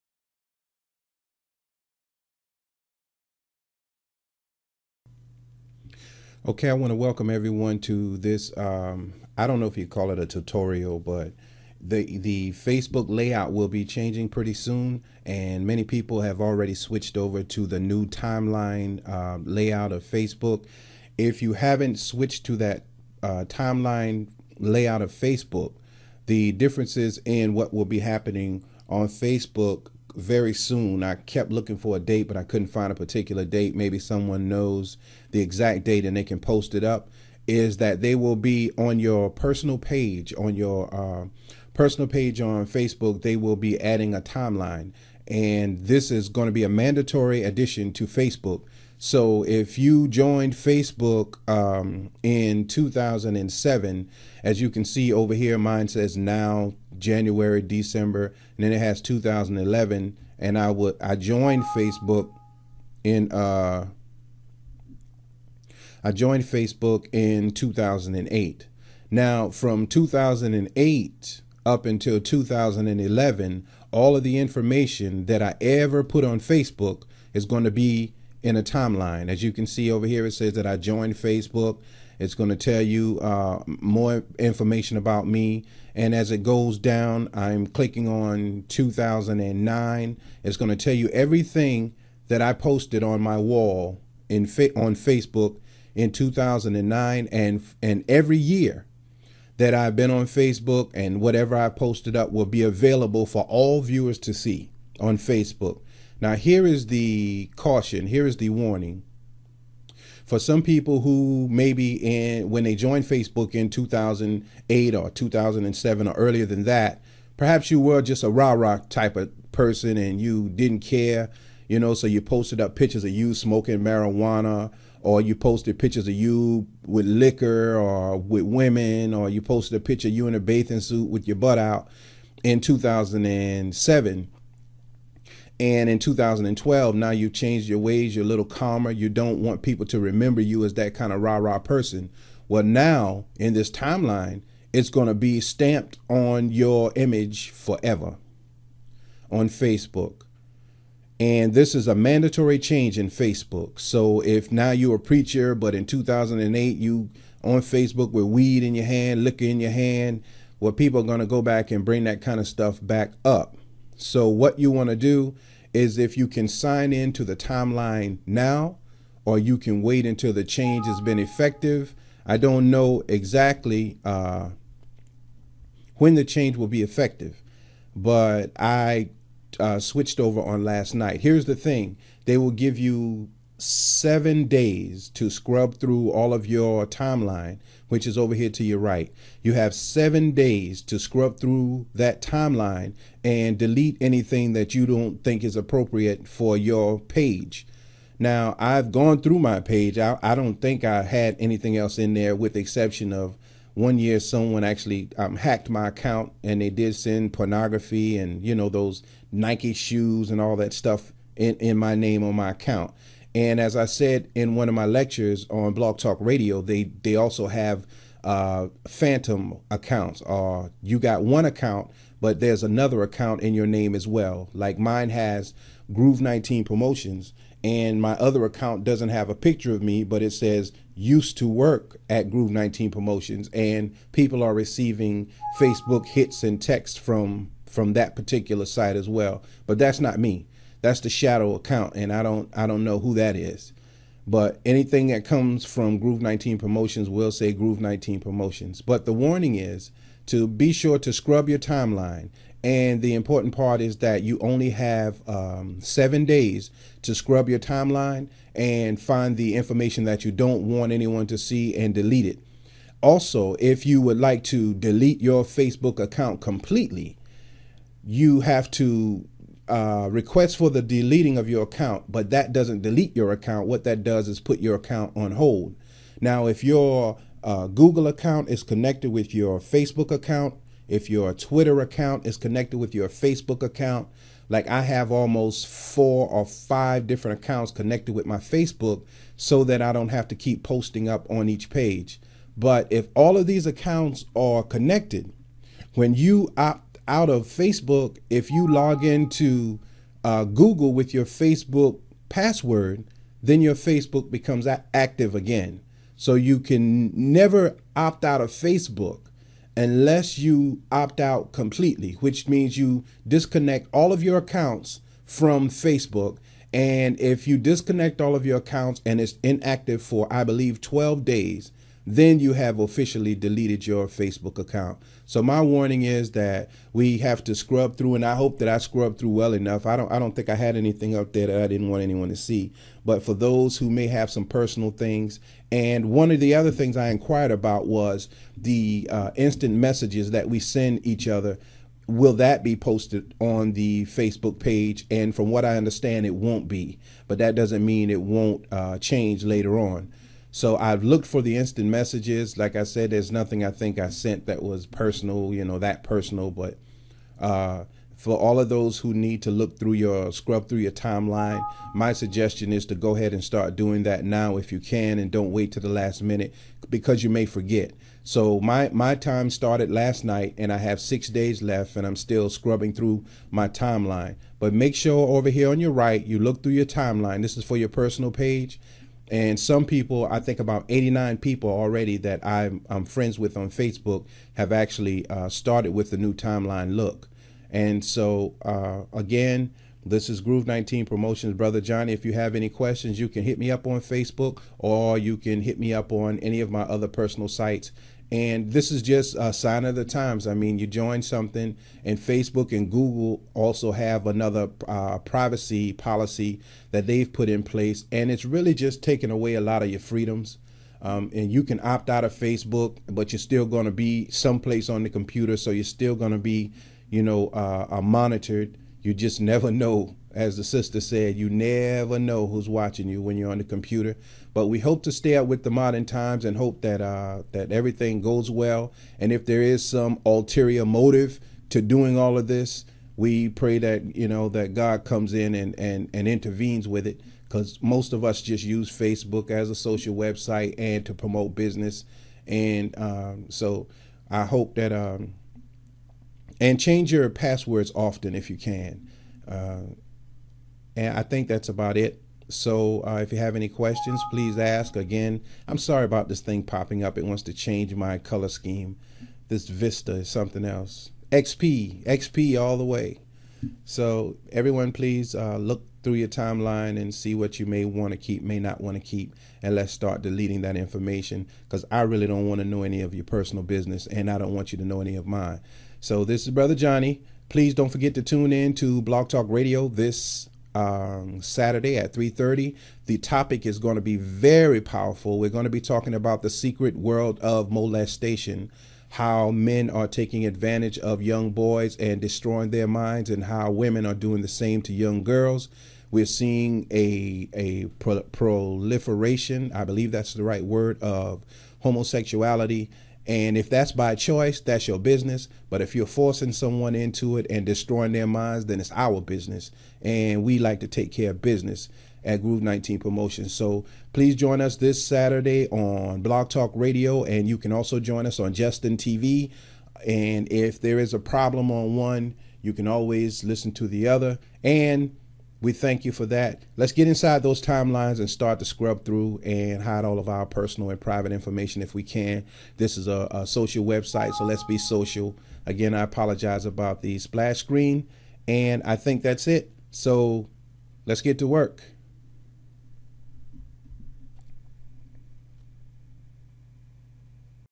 Urgent! New Changes To Facebook
The new changes soon to be made to the Facebook layout may prove to be a reminder of Bad/Good memories. The Timeline will show every post that you ever made to Facebook. Find out what you can do to protect your private information.